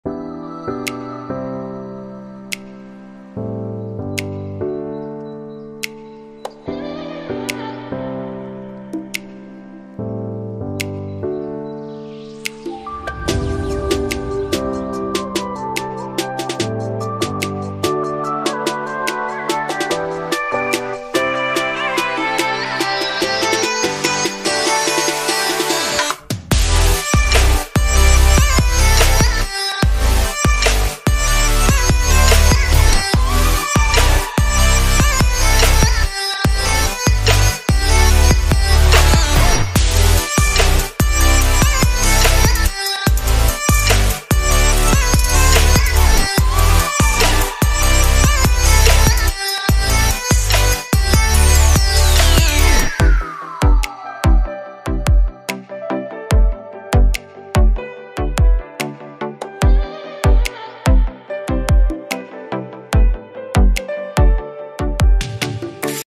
Votaciones - Modelo por Un Día | Participante No. 8
Ya puedes votar por tu favorita dando like a este video o tambien registrarte en nuestro instagram @diamondmodelsrd enviando un mensaje con tu nombre, edad y foto de tu preferencia.

La ganadora sera la que reciba la mayor cantidad de votos sumando las reproducciones + cantidad de likes y formara parte del desfile modelo por un dia con una beca.

Las votaciones cierran el 31 de diciembre a las 5:00pm hora de Republica Dominicana.